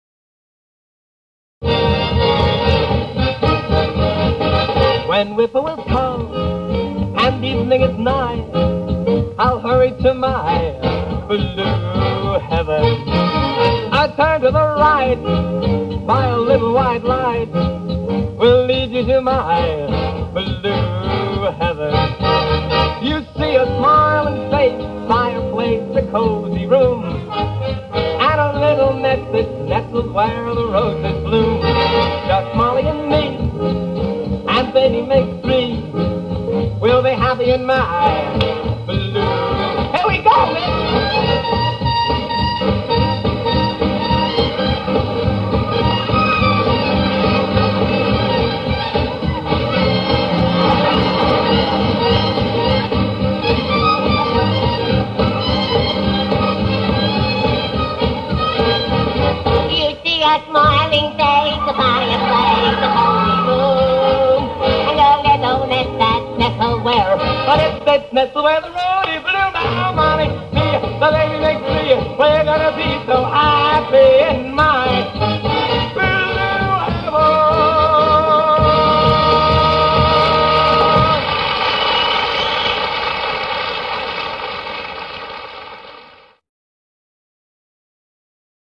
Comedian Morty Gunty Goes Crazy - 1955
Comedian Morty Gunty may have had his life cut short by cancer at age 55, but he managed to leave behind a legacy of very funny performances, including this early sample from 1955 when he was 26-years-old.